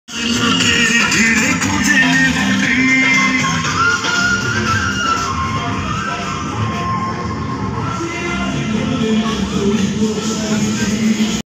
22 April 2022
সৈয়দ ফজলুল হক ডিগ্রি কলেজ শুভ নববর্ষ পহেলা

 বৈশাখে উদযাপিত অনুষ্ঠান